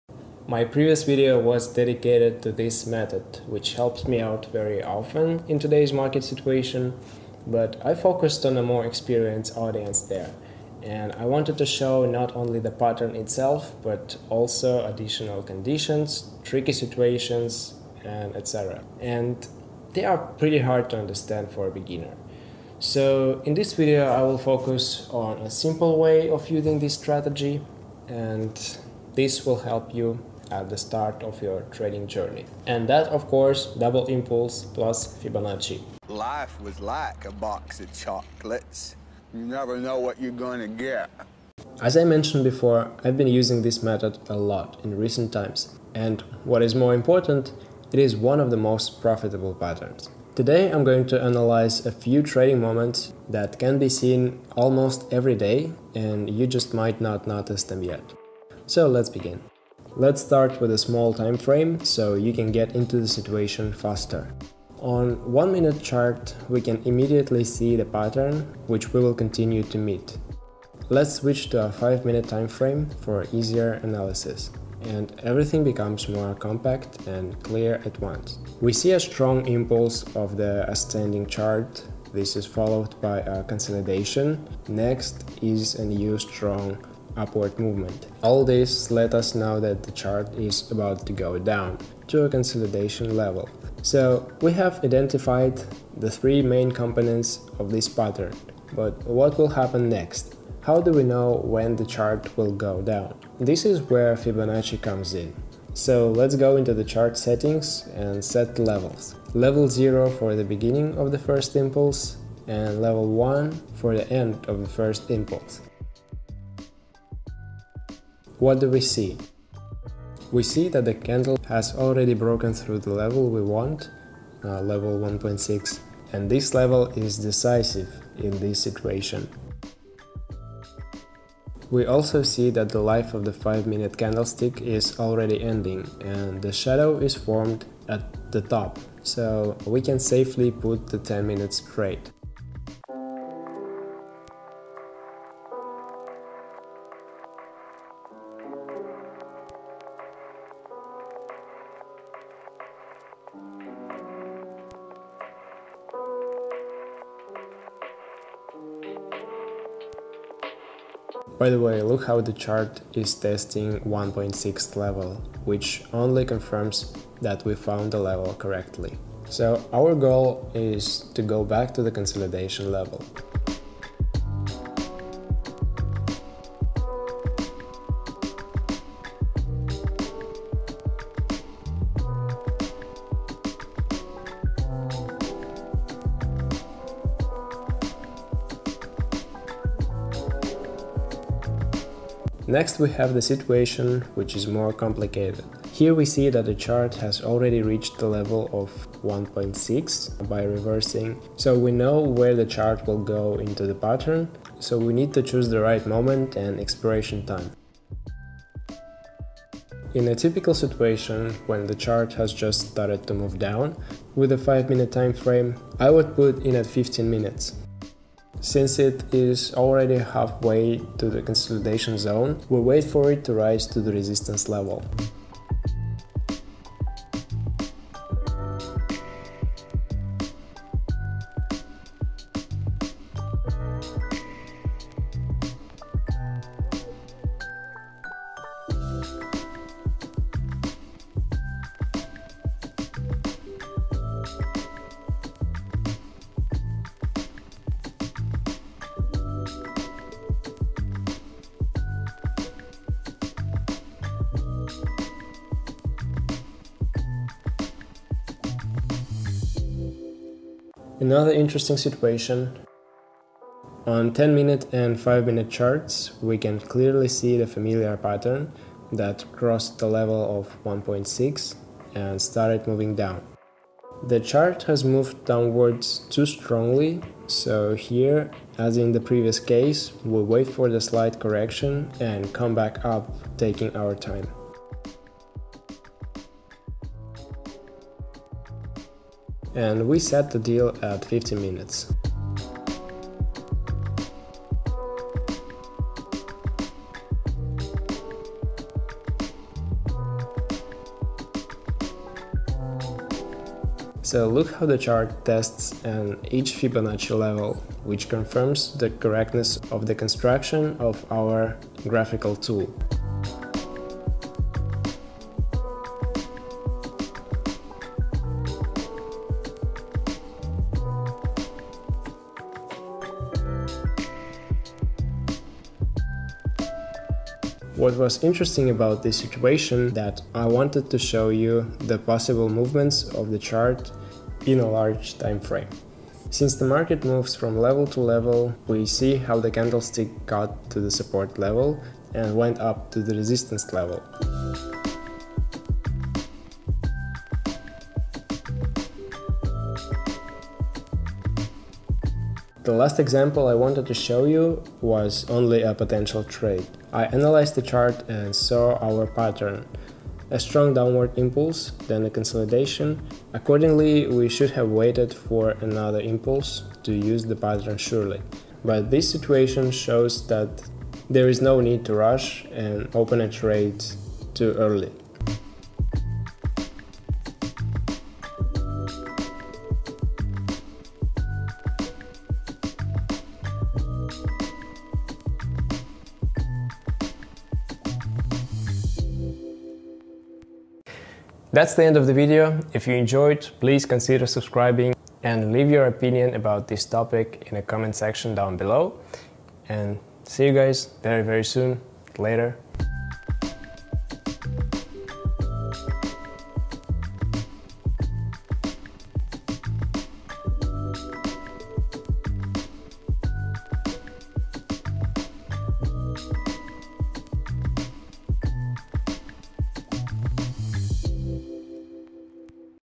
Best binary options strategy | Double Impulse trading strategy
In this video I'll show you a quick explanation of my favorite binary options strategy - double impulse trading strategy.